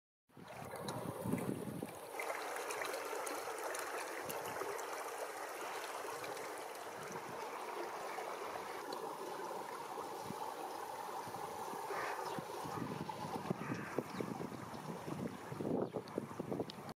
Longhorn Bee on Japanese Rose ハイイロヒゲナガハナバチ♀？がノイバラを訪花採餌
A female of longhorn bee (unidentified, possibly Eucera sociabilis, family Apidae) visiting white flowers of Japanese rose (Rosa multiflora, family Rosaceae) for nectar and pollen.  Early-June 2016 in Japan.

ノイバラの花で採餌するハイイロヒゲナガハナバチ♀？